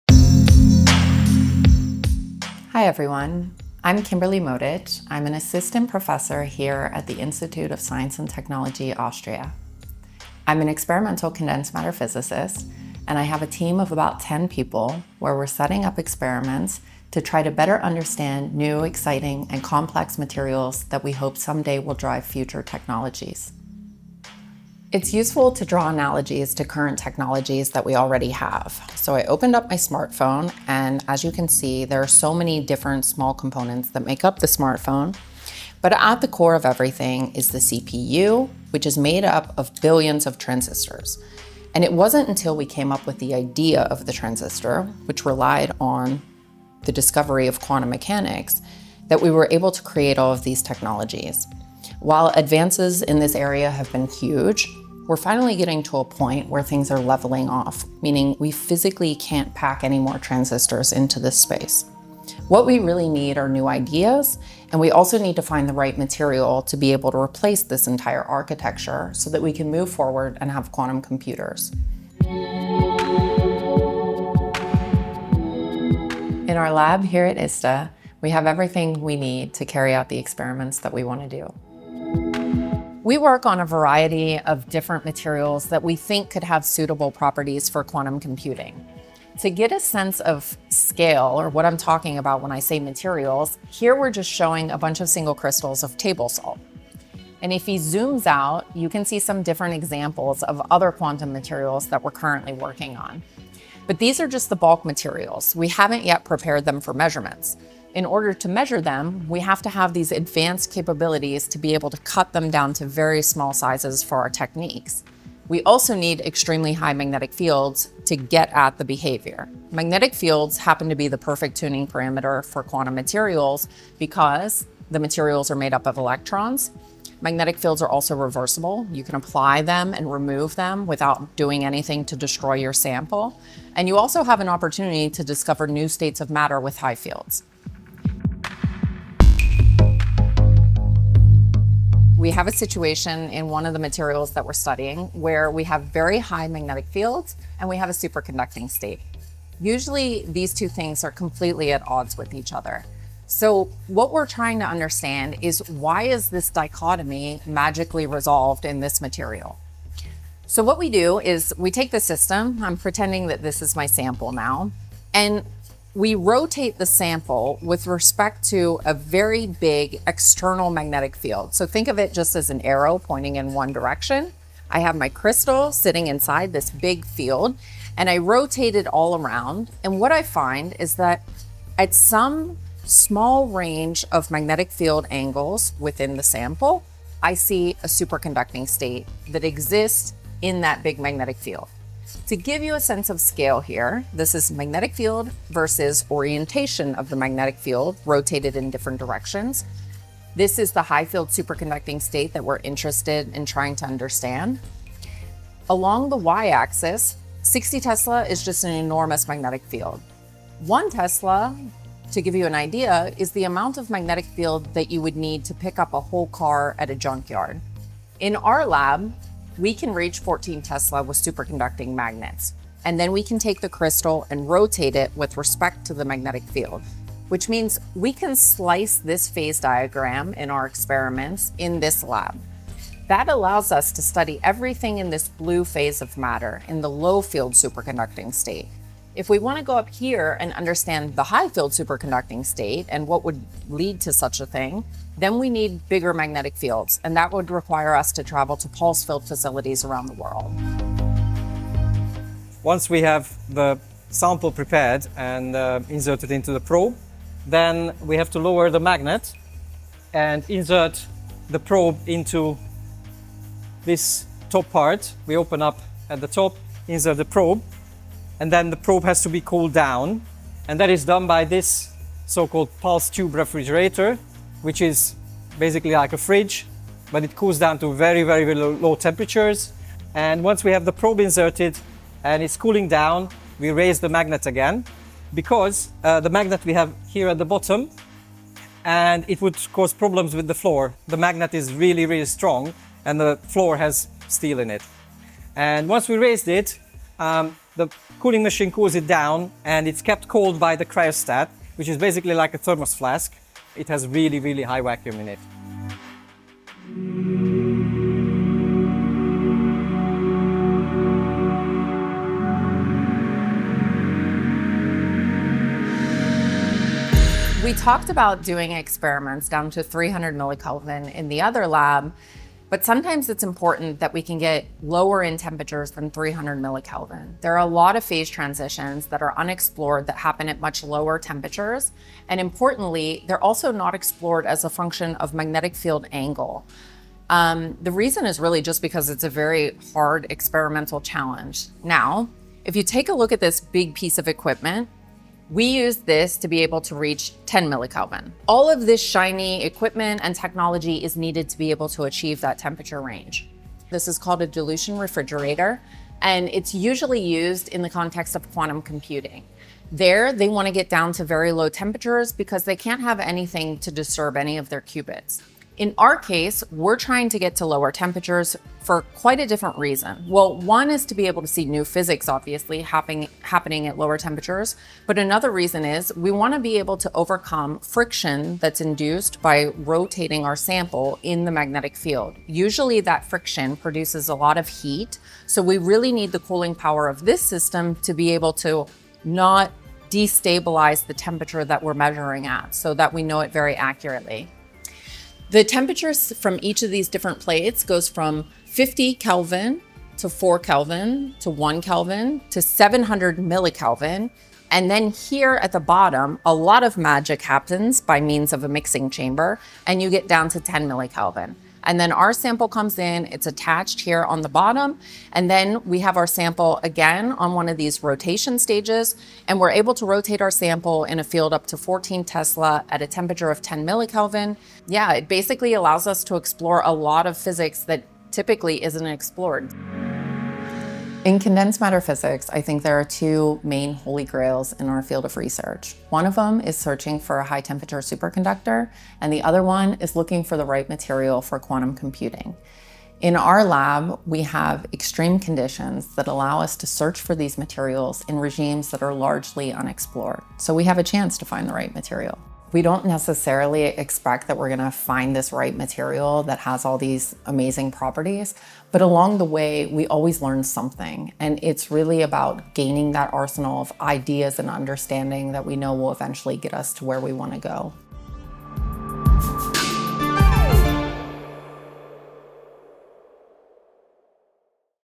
A Dive Into the Minuscule World of Quantum Science with Kimberly Modic
Have you ever wondered what ingenious methods, special equipment, and other treasures are hidden inside a lab that researches the special properties of quantum materials?

With her research group, ISTA physicist Kimberly Modic pushes the limits of today’s technologies and explores yet unknown quantum phenomena inside materials.

Follow her and her team on a behind the scenes tour of her lab and learn how they dive into the minuscule world of quantum science. A story of giant magnets, ultracold temperatures, and superconductivity at a microscopic scale in the 2025 International Year of Quantum Science and Technology.